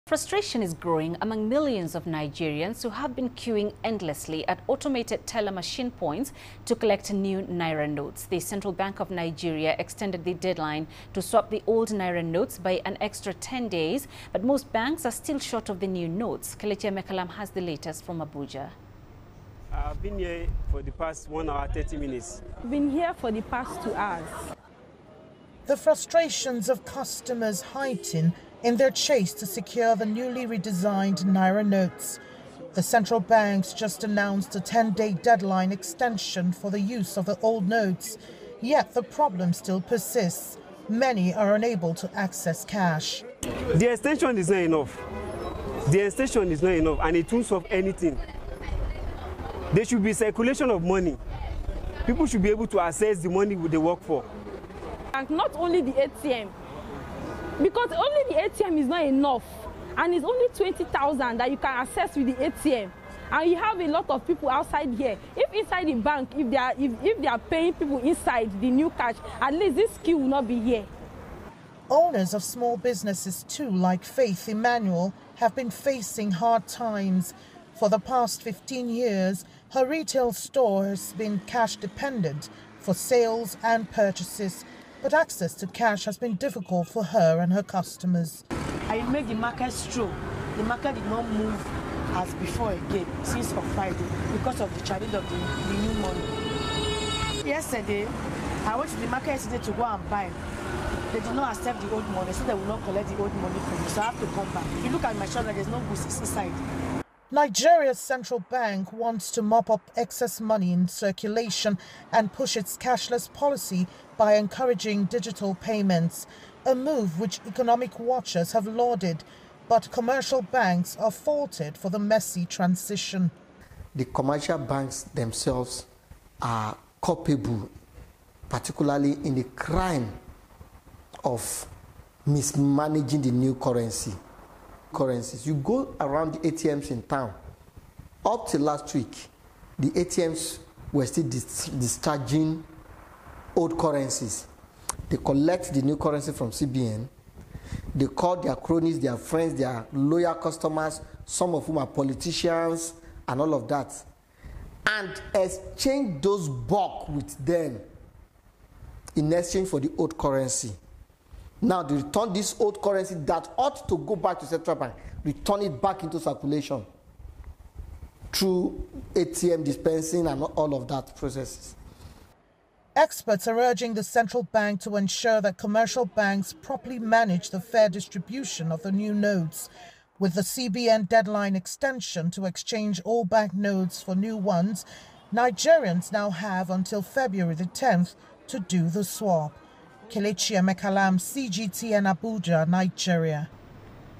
Nigerian banks face a shortage of new naira notes
Frustration is growing among millions of Nigerians who have queued at automated teller machines to collect new naira notes. The Central Bank of Nigeria extended the deadline to swap the old naira notes by an extra ten days, to February 10. But no one is confident that banks will have enough of the new notes by then.